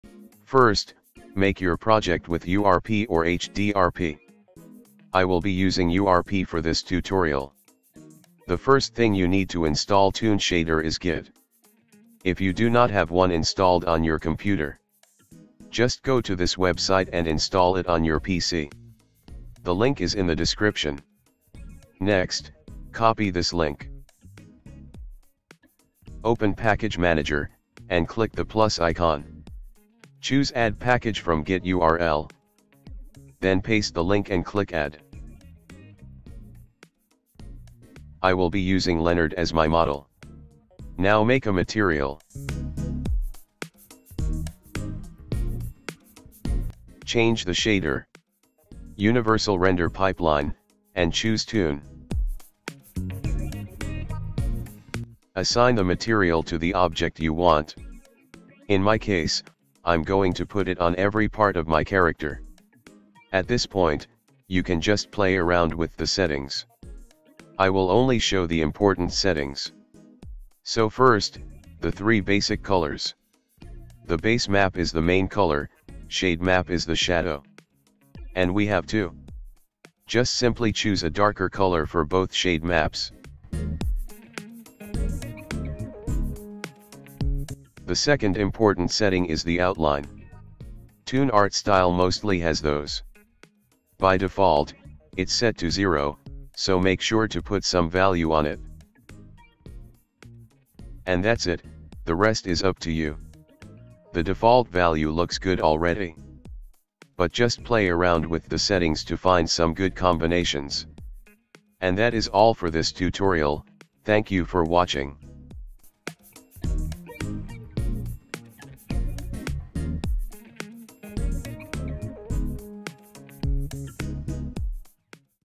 Unity tutorial - Unity Toon Shader (UTS3)
How to install unity toon shader (UTS3).
UTS3 is a toon shader for images and video that is designed to meet the needs of creators working on cel-shaded 3DCG animations. Unlike other pre-render toon shaders, all features can be adjusted in real-time on Unity, which is the greatest feature of Unity Toon Shader.

0:00 Use HDRP or URP
0:06 Install Git
0:21 Copy com.unity.toonshader
0:39 Create a material
1:09 3 basic colors
1:27 Outline
1:40 Outro